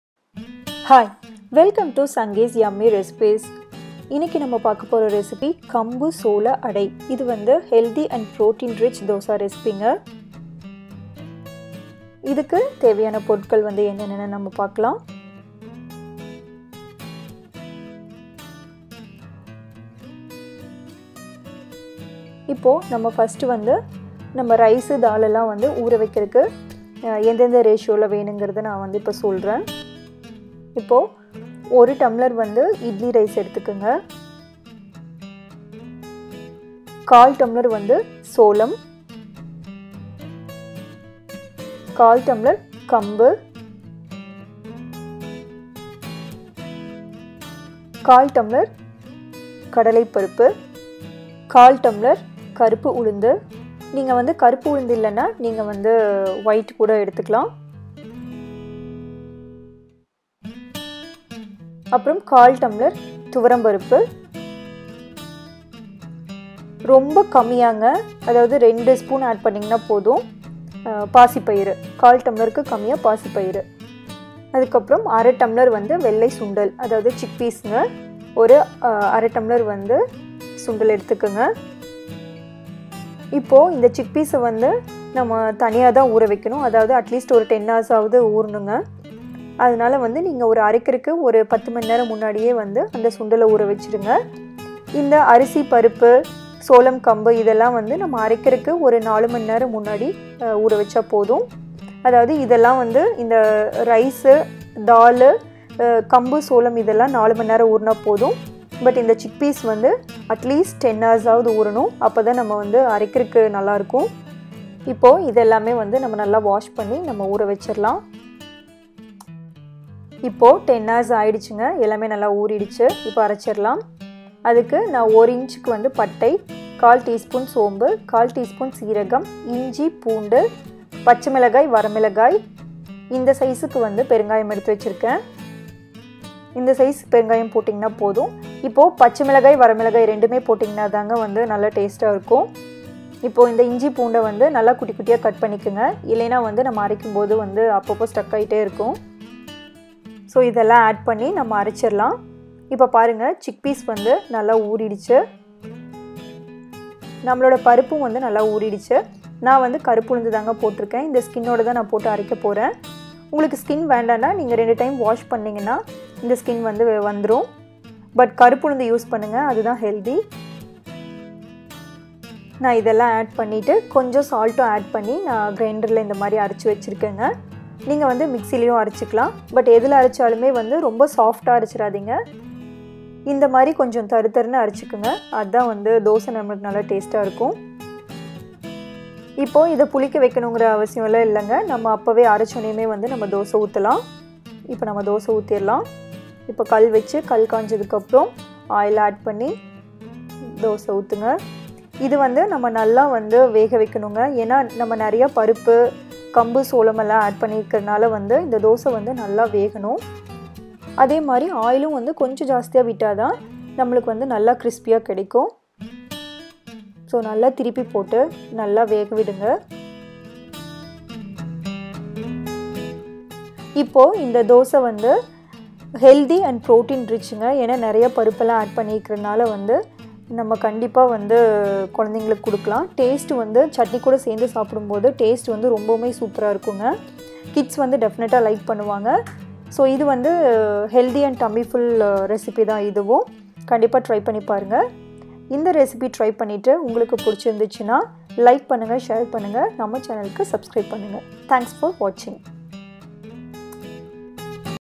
Cholamkambu Adai Dosai | Healthy millet adai dosai | Tasty millet dosai
Hello friends,

                 Please watch this dosai recipe which is tasty and protein rich and it's tummy full recipe too. we always interested in finding new healthy and tasty recipes with care. try this protein rich adai recipe and let me know in comment section. Thank you

Ingredients

 Idli rice   1 cup
 Sorghum/Cholam  1/4 cup
 Pearl millet/kambu  1/4 cup
 Channa dal  1/4 cup
 Black urad dal  1/4 cup
         Chickpeas        1/4 cup
         Ginger           2 inch(finely chopped)
         Garlic           5 nos.(finely chopped)
         Asafoetida         1/4 inch size
         green & red chillies          4 nos.